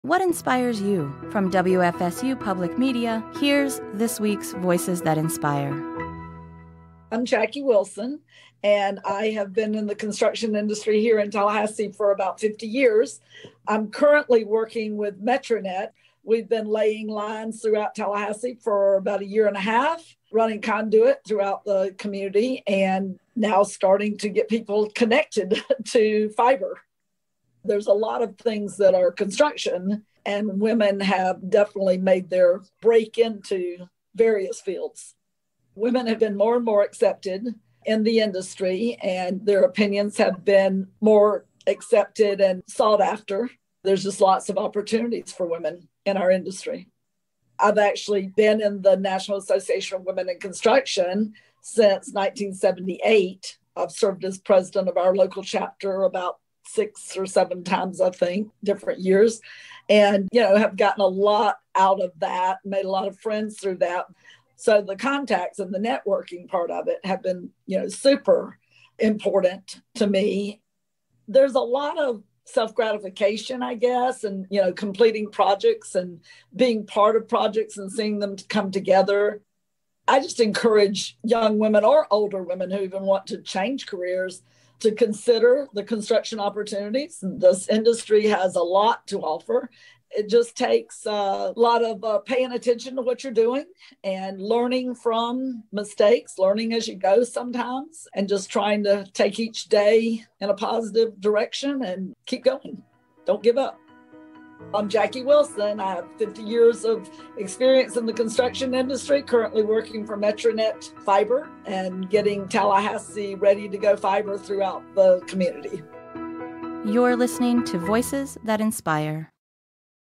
Jackie Wilson- Keep Going. Don't Give Up.
Jackie Wilson works with MetroNet and has over 50 years of experience in the construction industry. She’s been a member of the National Association of Women in Construction since 1978. She believes there are unexplored opportunities for women in her field.